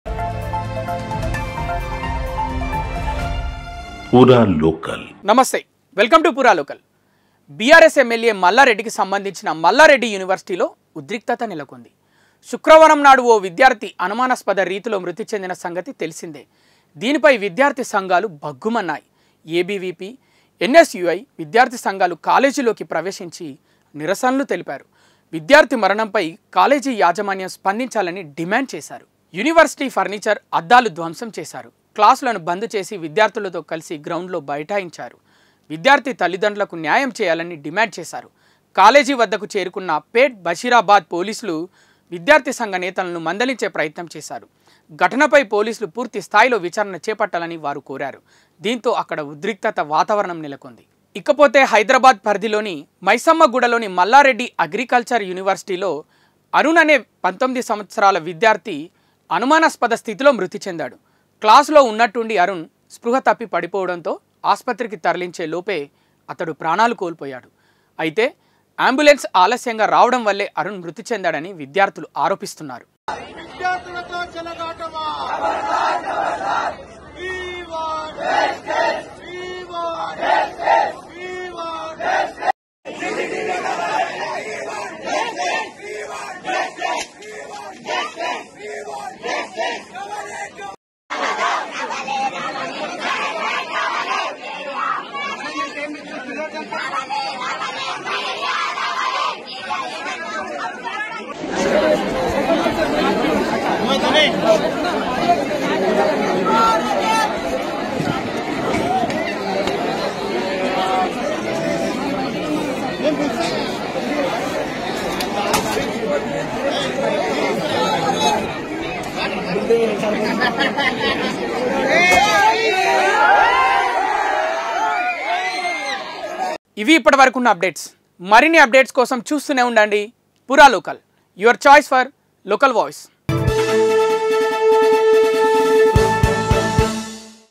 మల్లారెడ్డి యూనివర్సిటీ లో ఫర్నిచర్, అద్దాలు ధ్వంసం..!! | Mallareddy University Issue | Pura Local
Mallareddy University Issue